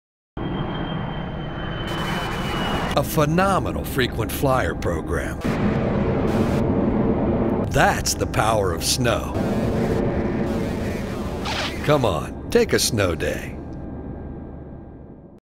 The Power of Snow - "Frequent Flyer - A"
Winter 2009-'10 TV Campaign for Liberty Mountain Resort, Ski Roundtop and Whitetail Resort.  Witness the Power of Snow yourself...and, Take a Snow Day!